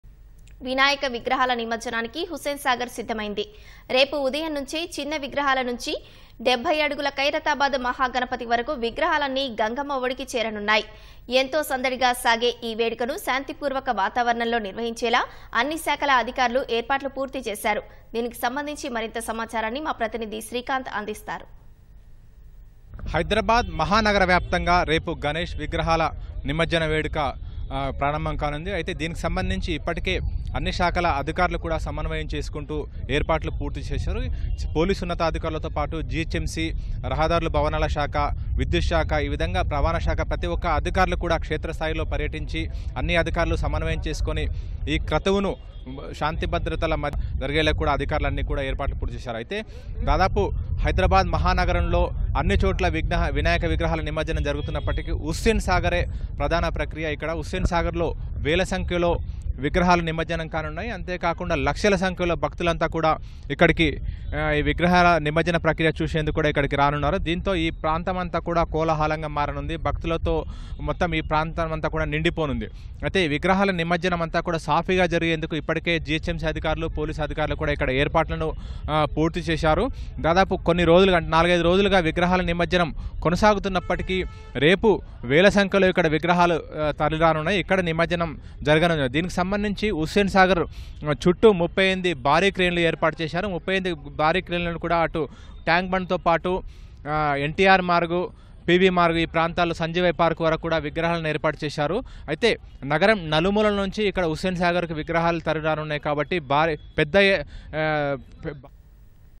All Set For Ganesh Immersion Idols in Hussain Sagar | వినాయక నిమజ్జనానికి హుస్సేన్‌ సాగర్‌ సిద్ధం
వినాయక విగ్రహాల నిమజ్జనానికి హుస్సేన్ సాగర్ సిద్ధమైంది. రేపు ఉదయం నుంచే చిన్న విగ్రహాల నుంచి 70 అడుగల ఖైరతాబాద్ మహాగణపతి వరకు విగ్రహాలన్నీ గంగ ఒడికి చేరనున్నాయి. ఎంతో సందడిగా కొనసాగే ఈ వేడుకను శాంతిపూర్వక వాతావారణంలో నిర్వహించేలా అన్ని శాఖల అధికారులు ఏర్పాట్లు పూర్తి చేశారు.